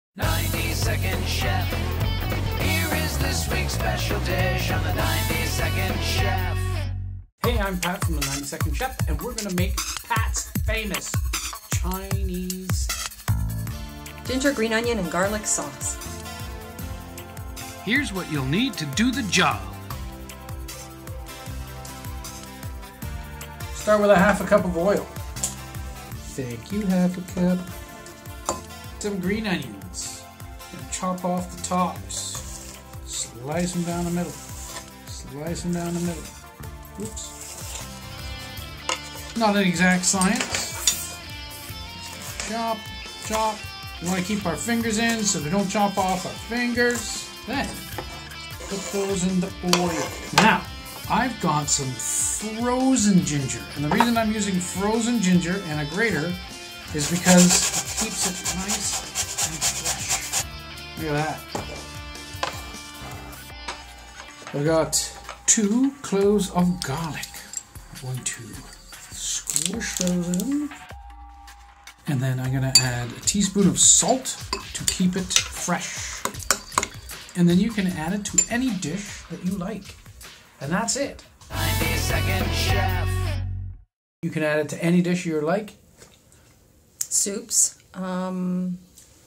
90 Second Chef | S1 - EP7: Chinese Ginger, Green Onion and Garlic Sauce
Learn to cook in 90 seconds, This Simple sauce will make any savory dish taste great